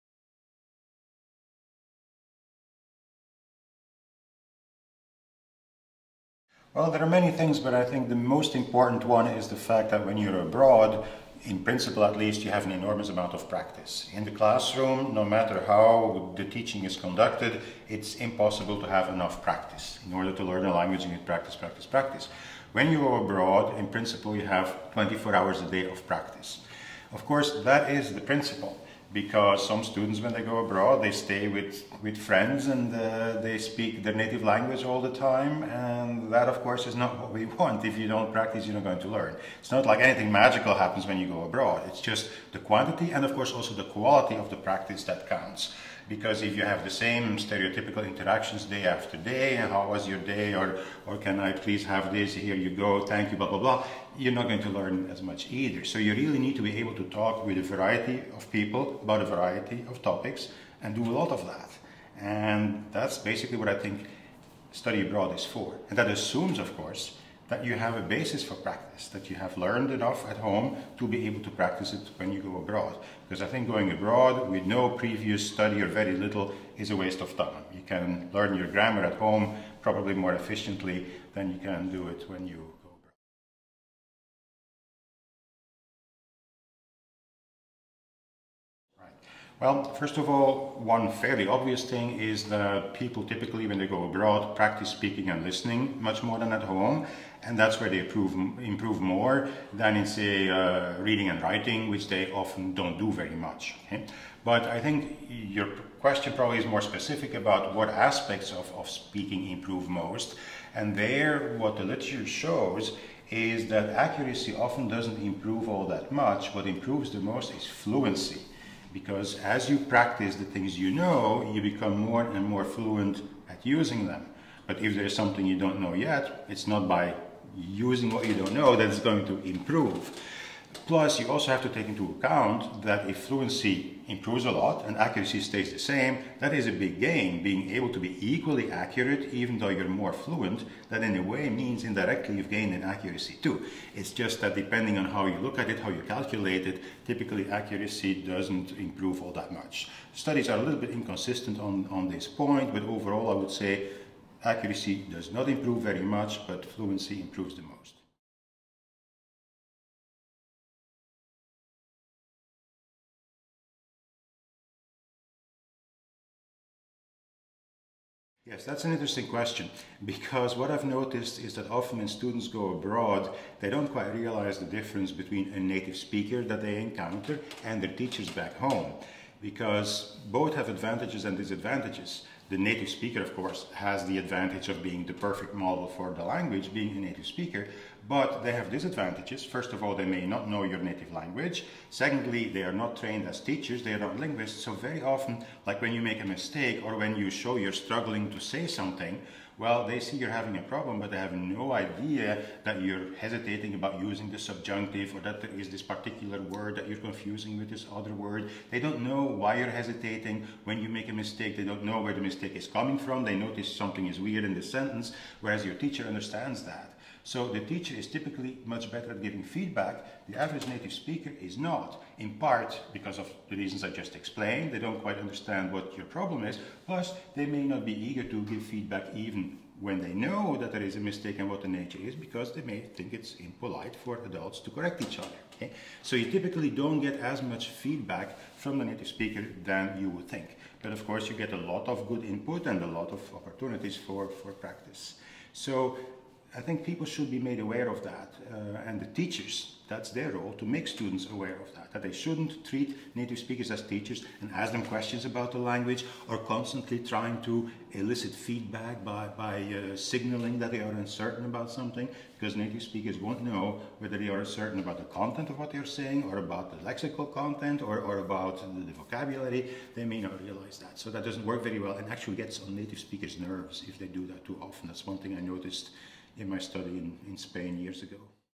Robert de Keyser SA 1
Speaker: Robert de Keyser

Question 1. From the point of view of language acquisition, what differentiates SA from FI?

Question 2. What skills improve during SA?

Question 3. What advice would you give to teachers and students to best prepare and to benefit most from the SA period?